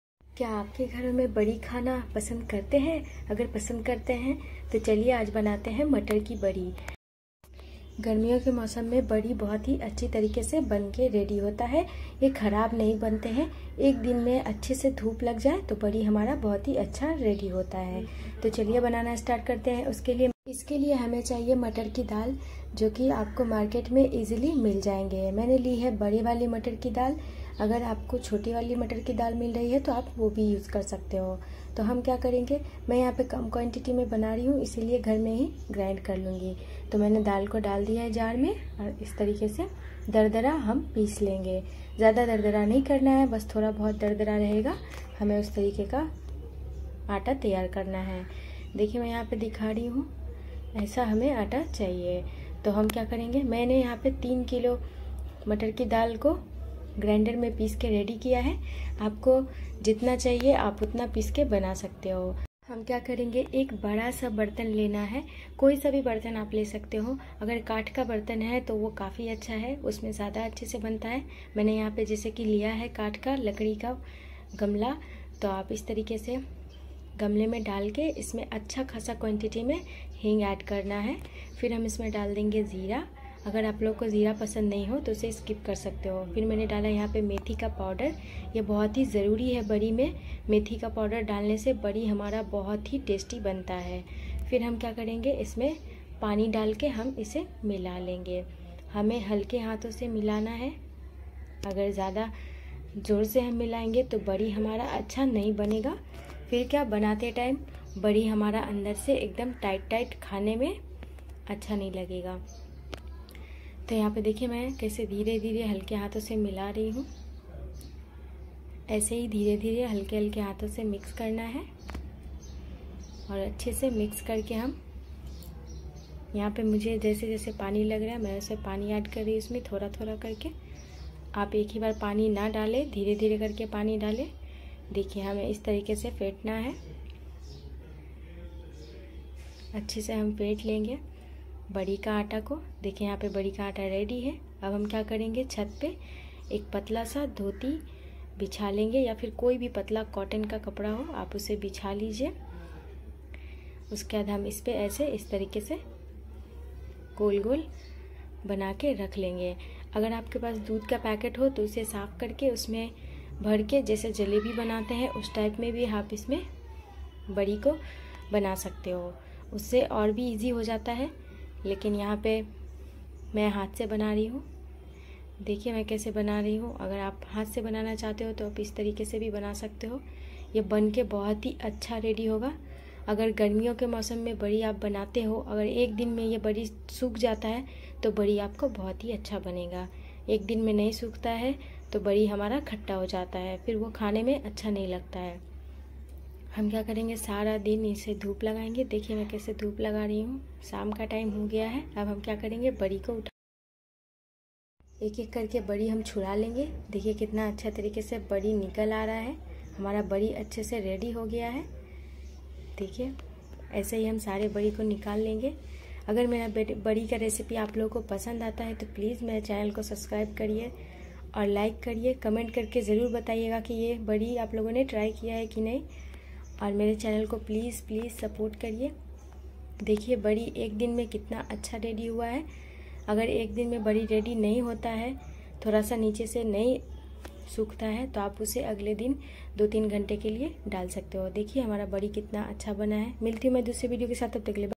ऐसे बनाएं मटर दाल बड़ी कभी भी खराब नहीं होगी सालो साल store kar के rekhe l
ऐसे बनाएं मटर दाल बड़ी कभी नहीं होगी खराब सालो साल store kar ke rakhe l

ingredient:

Mater dal
Methi pawder 2tsp
3 Tbsp hing
jeera seeds 2tsp
water